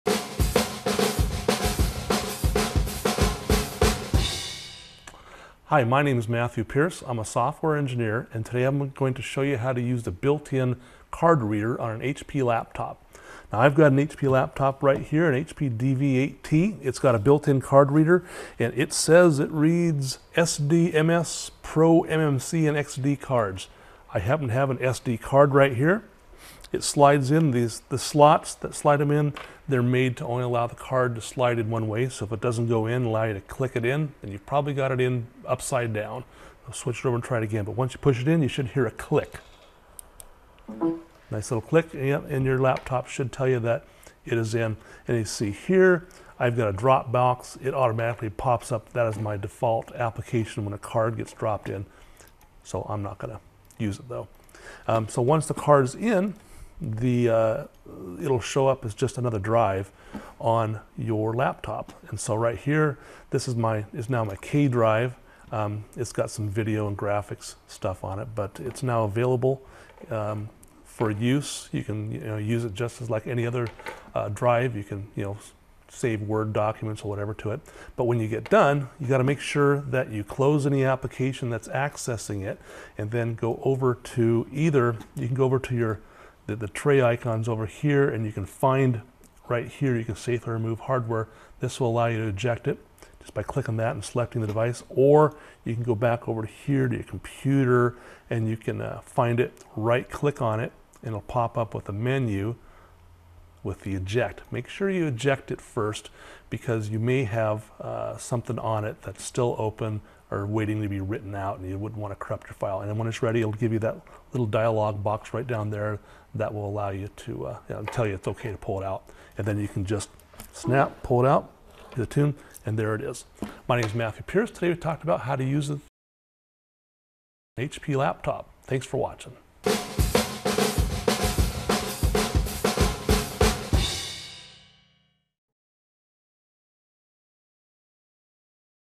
How to Use the Built-In Card Reader on an HP Laptop
How to Use the Built-In Card Reader on an HP Laptop. Part of the series: Computer Help & Tips. The built-in card reader on an HP laptop is a great way to access information from sources like digital camera cards on your PC. Use the built-in card reader on an HP laptop with help from a software engineer with broad and extensive experience in this free video clip.